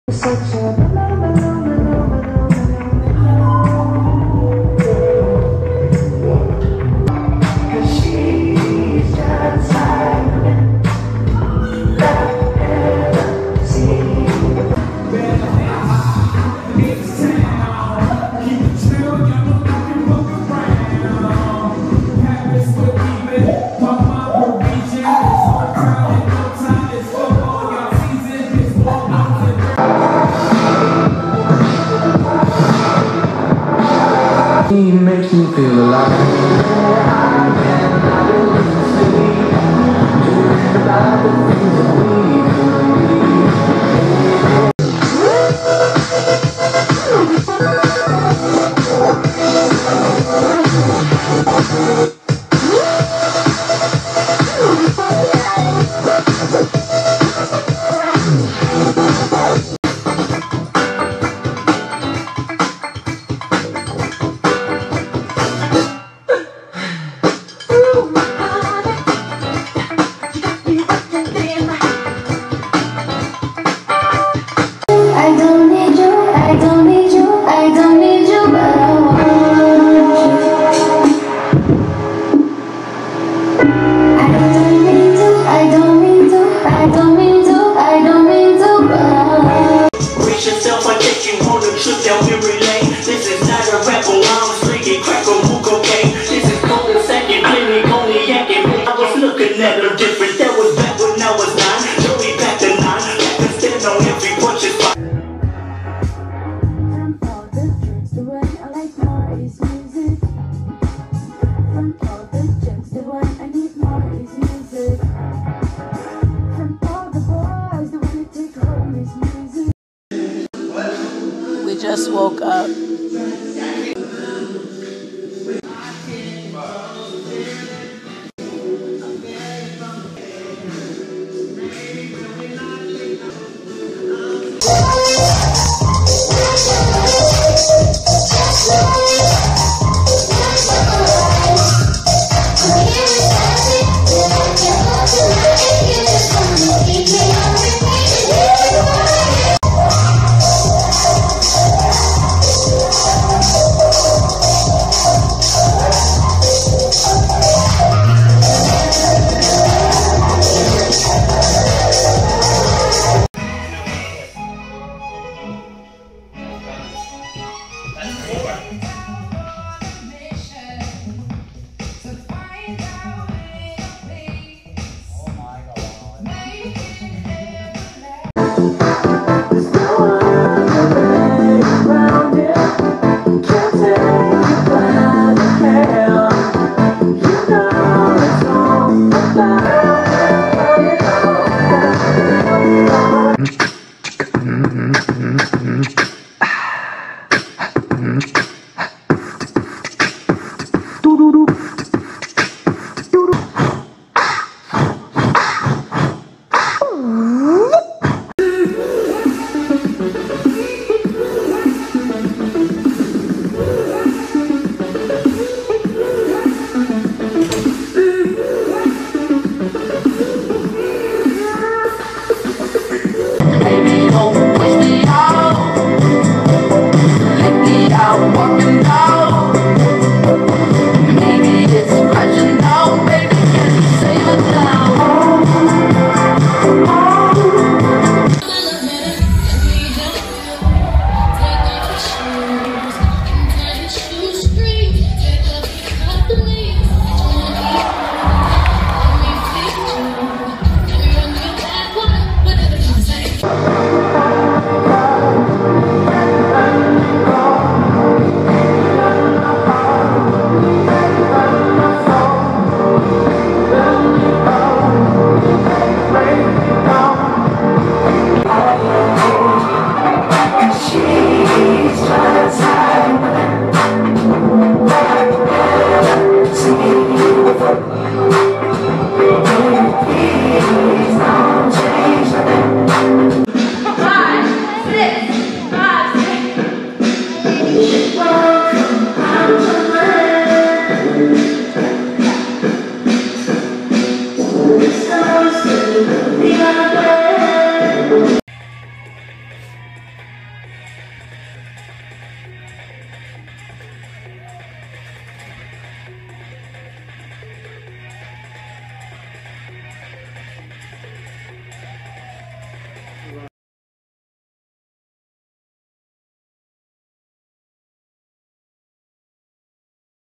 IG Compilation
Lately I've been getting a few people, anons, etc., telling me I should upload a collage of all my IG videos. I never thought to do that.. I post very frequently on my Instagram, & leave my YouTube channel for more of the "production" type stuff. I figured I'd post the videos with the most likes & videos I forgot about that give me such good memories. Well, enjoy. I'll try to post more on here.